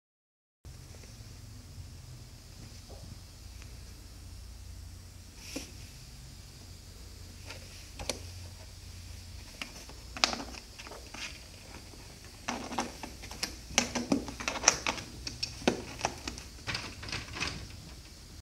How to Climb out of a Deep Narrow Box | Tabby Cat
Demonstrated to you by Fernando.
This video was recorded in March, 2019.

All our short videos we dedicate to our dear friend @Lisa Augustitus ❤